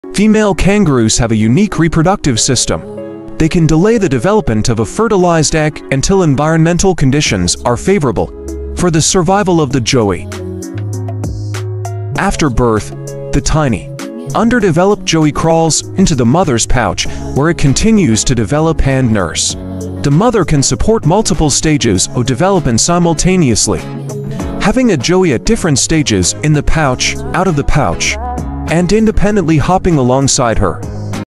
Unique reproductive system of kangaroos#shortsyoutube
Dive into the fascinating world of kangaroo reproduction in our latest video, "Nature's Marvels: Unlocking the Secrets of Kangaroo Reproduction." Discover the unique and awe-inspiring features of the kangaroo's reproductive system, a marvel of adaptation in the animal kingdom.

Join us as we explore the intricacies of kangaroo reproduction, from their extraordinary reproductive anatomy to the specialized adaptations that make them one of nature's most fascinating species. Learn about the incredible phenomenon of embryonic diapause, a reproductive strategy that allows kangaroos to navigate the challenges of their environment with unmatched efficiency.

Our video takes you on a journey through the life cycle of kangaroos, shedding light on how these marsupials have evolved to thrive in their native habitats. Gain a newfound appreciation for the wonders of nature as we unravel the mysteries behind the kangaroo's reproductive prowess.

If you're curious about the marvels of the animal kingdom and want to witness the unique strategies that contribute to the survival of kangaroos, don't miss "Nature's Marvels: Unlocking the Secrets of Kangaroo Reproduction."